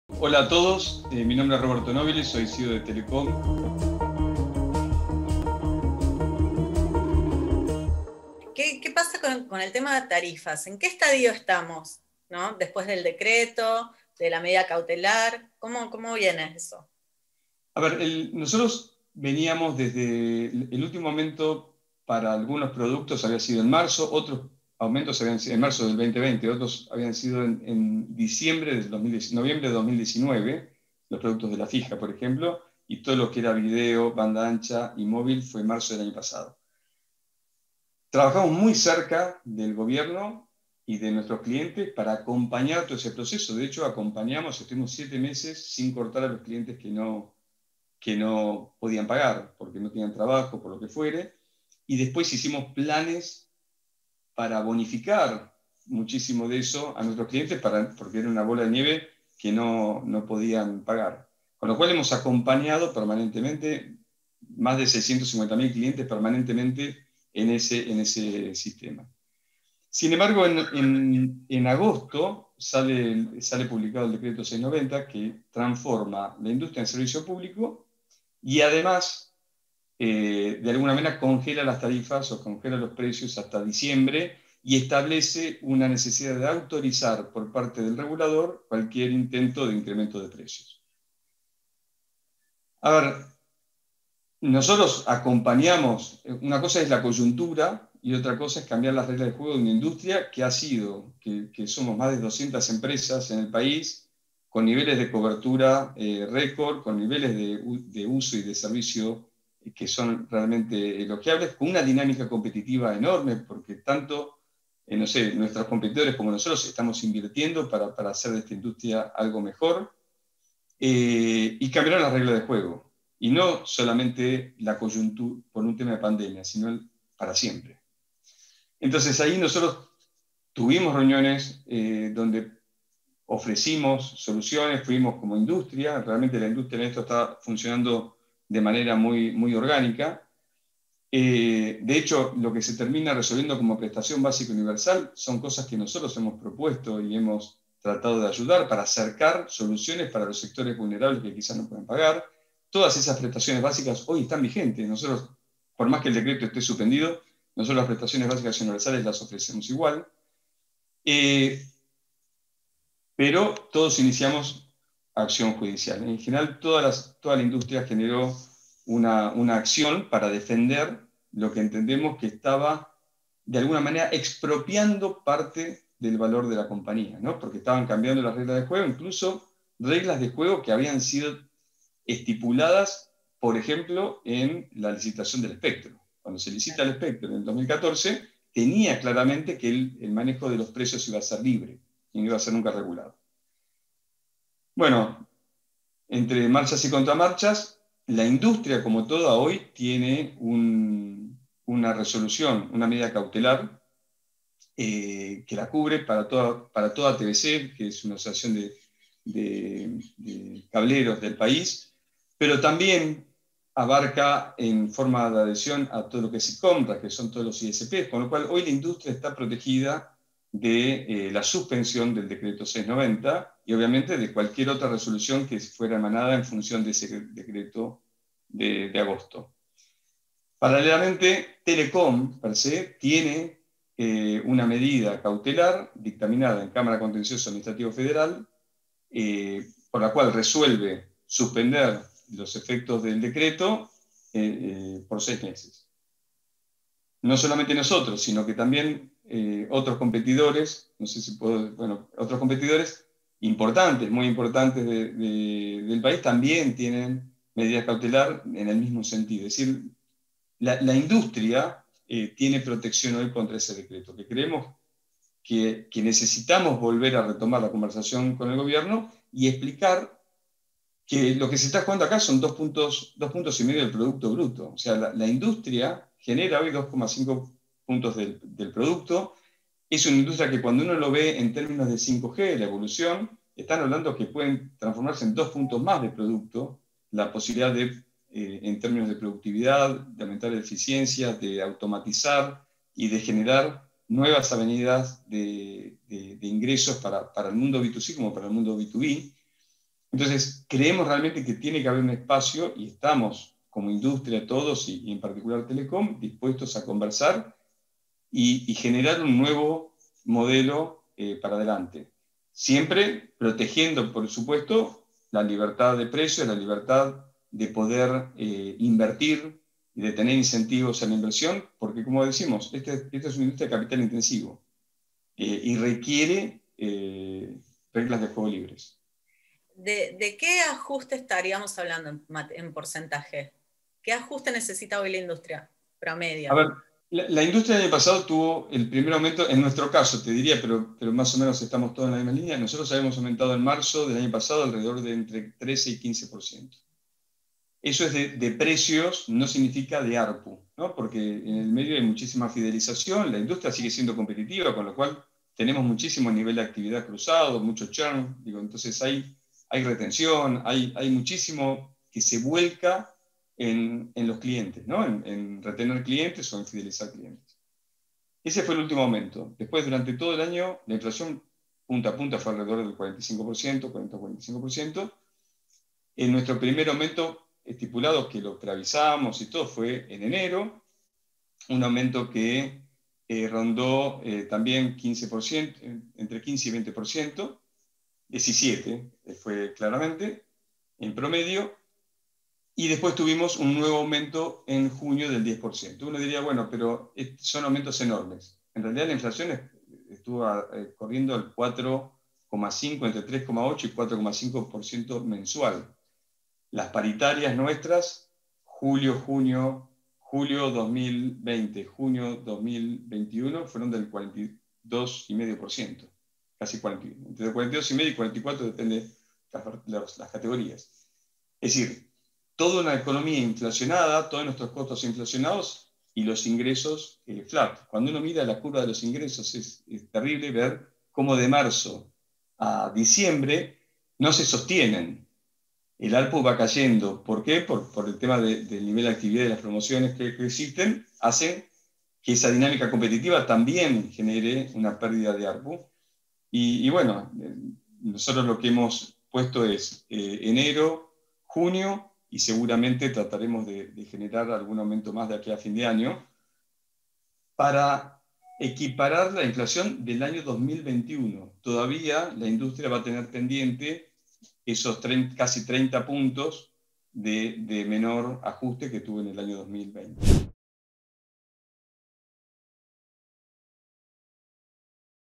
Exclusivo | Para el CEO de Telecom, el Gobierno pone en juego 4,5 puntos del PBI al regular tarifas
Roberto Nobile, CEO de Telecom, conversó en exclusiva con Bloomberg Línea sobre inversiones, red 5G y el impacto al mercado de las telecomunicaciones tras haber sido declaradas como servicio esencial